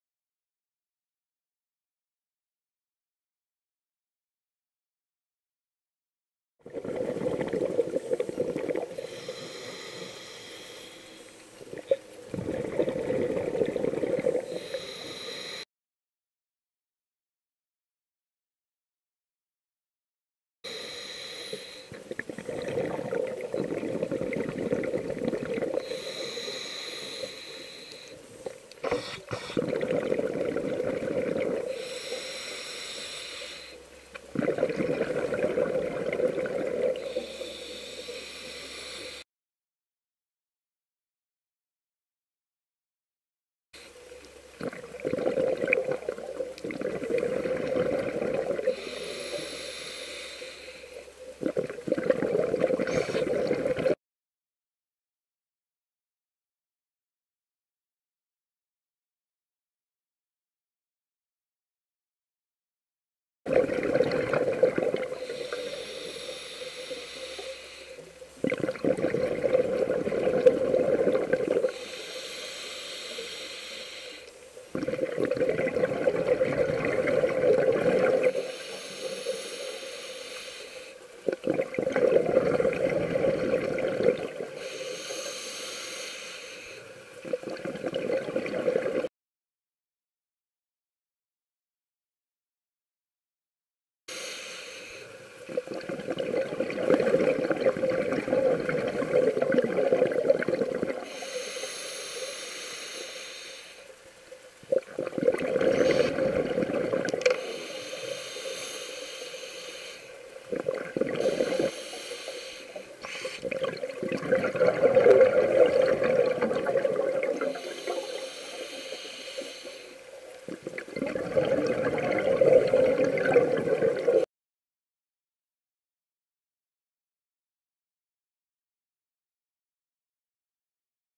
Dragonfly in Action - Pittwater Wreck, Australia
Underwater footage of a wreck filmed by Ben and the team at Raymarine Australia and imaged by the new Raymarine Dragonfly. This wreck is in Pittwater, NSW, near Sydney, and as you can clearly see, is a haven for the local fish.